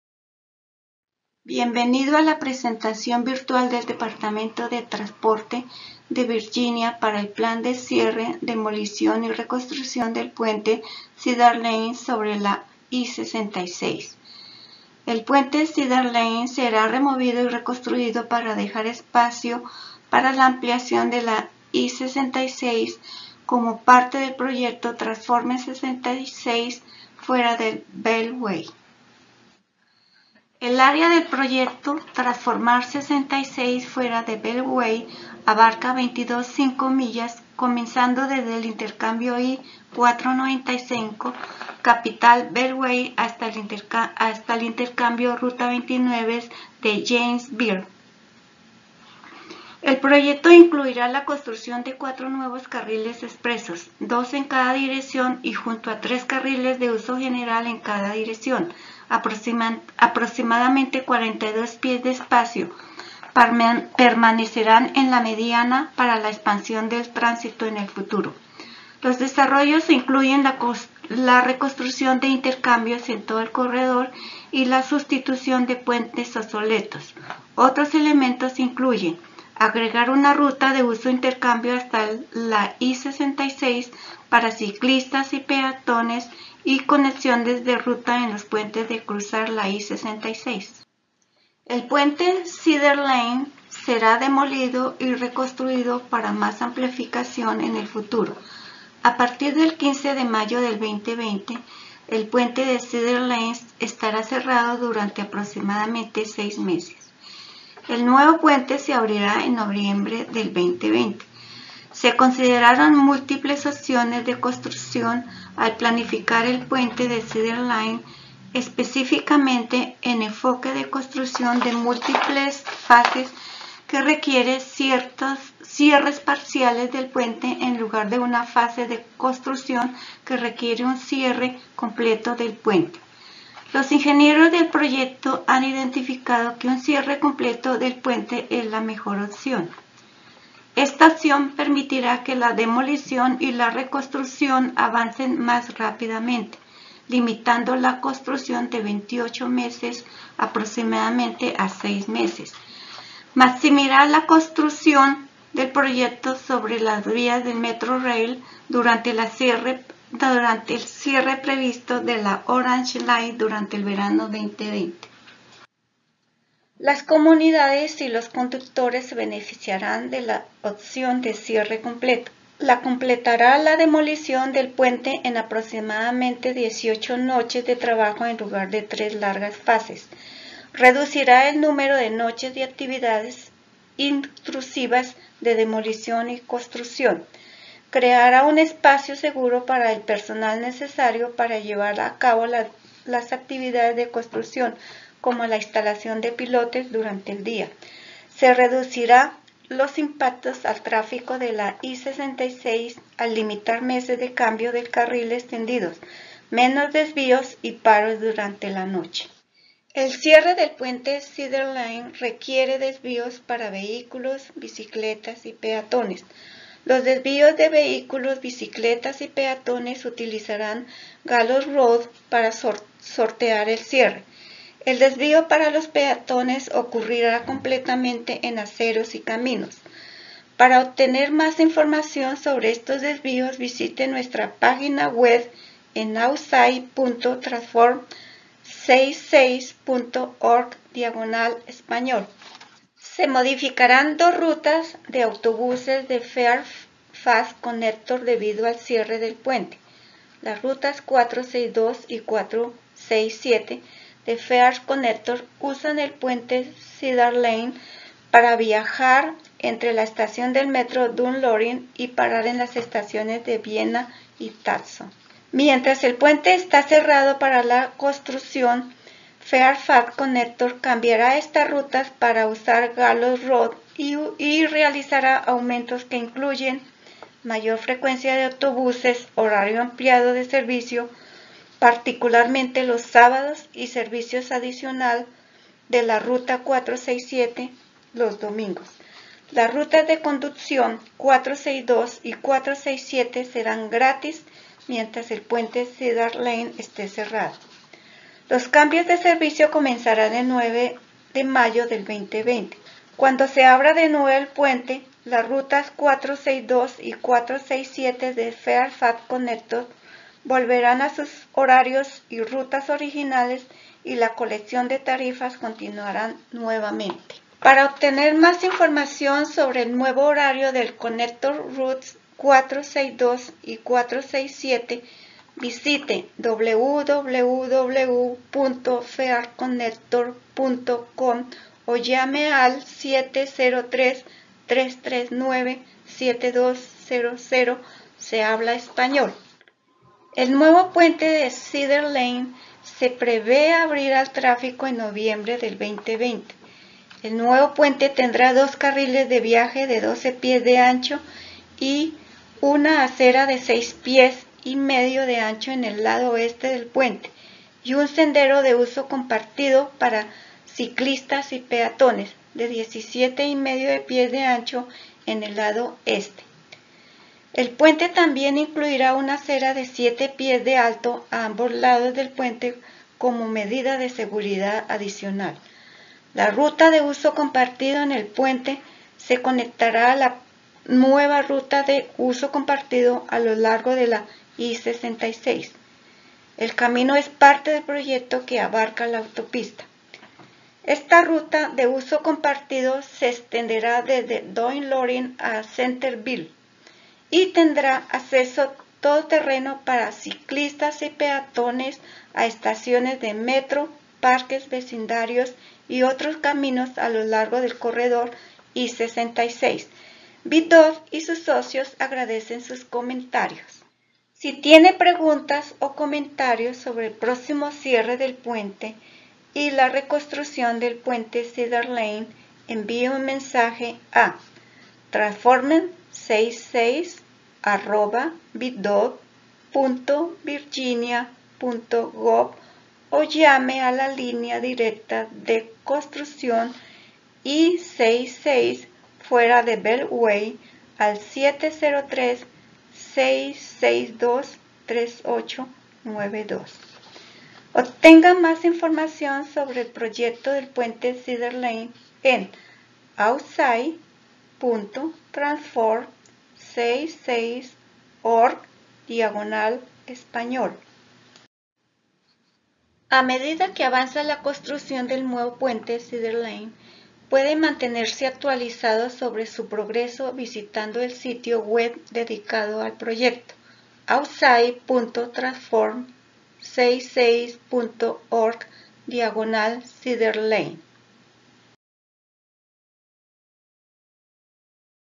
Cierre del Puente de Cedar Lane Sobre la I-66
Transformar 66 Fuera del Beltway El puente de Cedar Lane sobre la I-66 debe ser demolido y reconstruido para dejar espacio para la ampliación de la I-66 como parte del proyecto Transform 66 Outside the Beltway. Se planea cerrar el puente durante aproximadamente seis meses a partir del 15 de mayo de 2020. Los conductores, peatones y ciclistas deben seguir los desvíos publicados para cruzar la I-66 usando Gallows Road. Se espera que el nuevo puente de Cedar Lane se abra en noviembre de 2020. Para obtener más información, visite